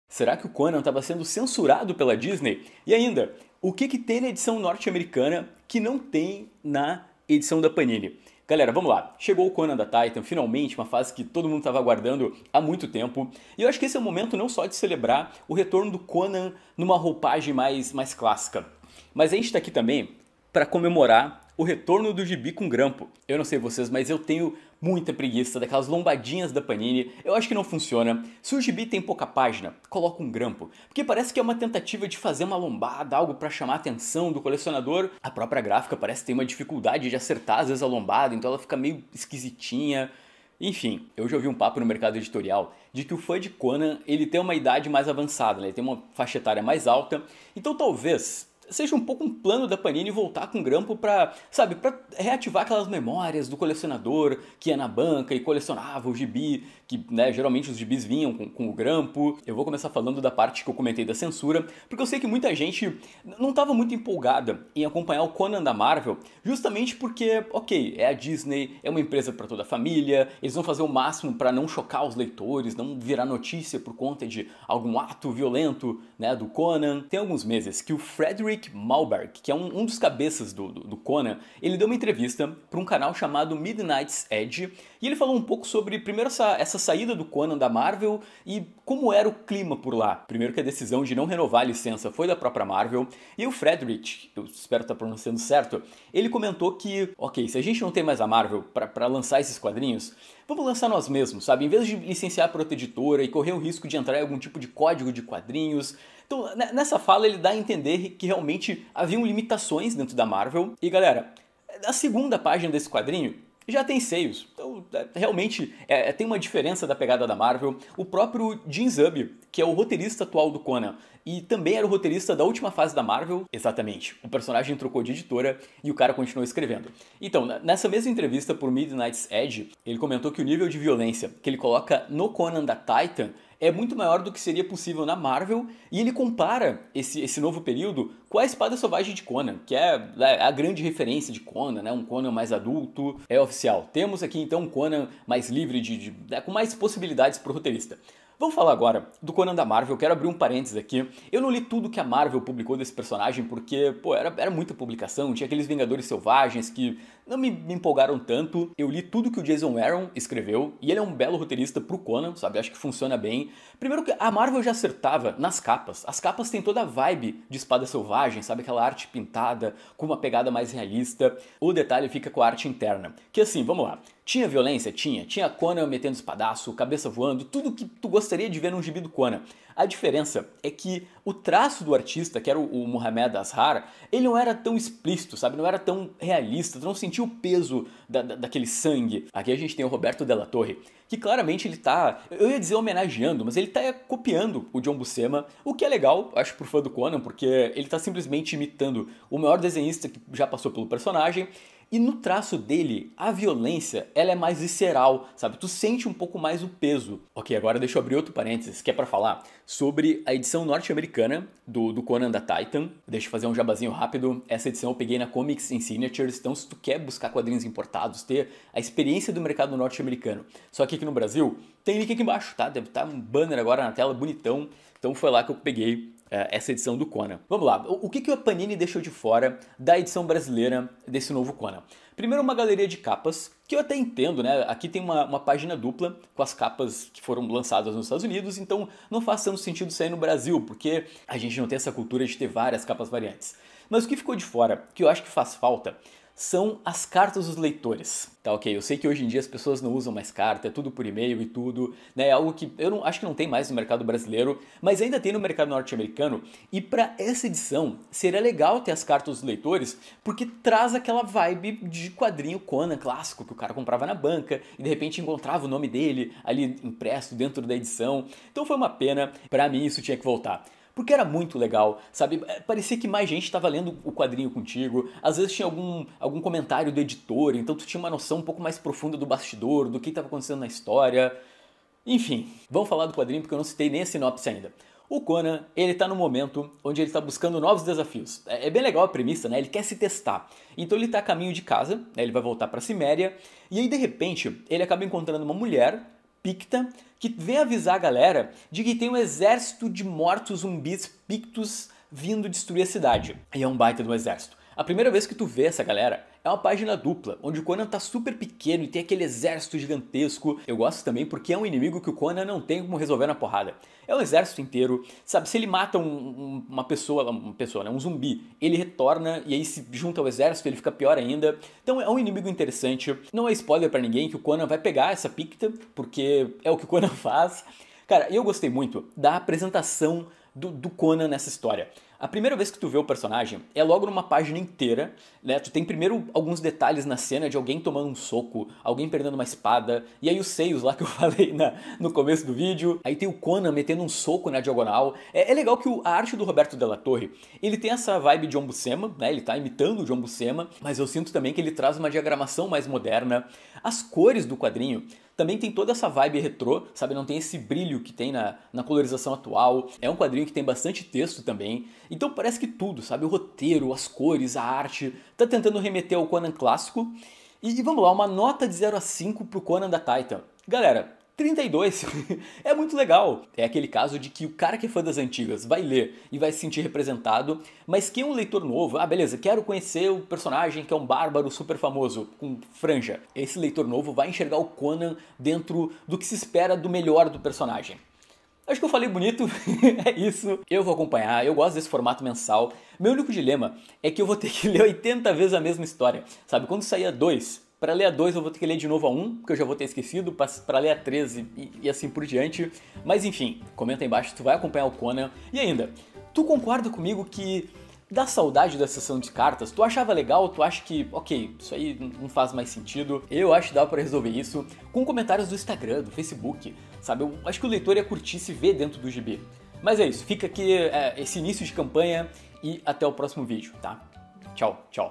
CONAN SEM DISNEY, O GRAMPO E A NOVA MENSAL
Links comentados!

SINOPSE OFICIAL
O lendário Conan de Robert E. Howard está de volta em uma nova série! Anos após a batalha de Venarium, um Conan cansado retorna à sua terra natal em busca de descanso e solidão. No entanto, um misterioso batedor chega para alertar os cimérios sobre uma ameaça iminente que marcha vinda da região selvagem dos pictos. Caberá a Conan e seu novo aliado conter essa nova horda de invasores.

Conteúdo original: Conan the Barbarian (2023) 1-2

Entre na 2quadrinhos Book Store!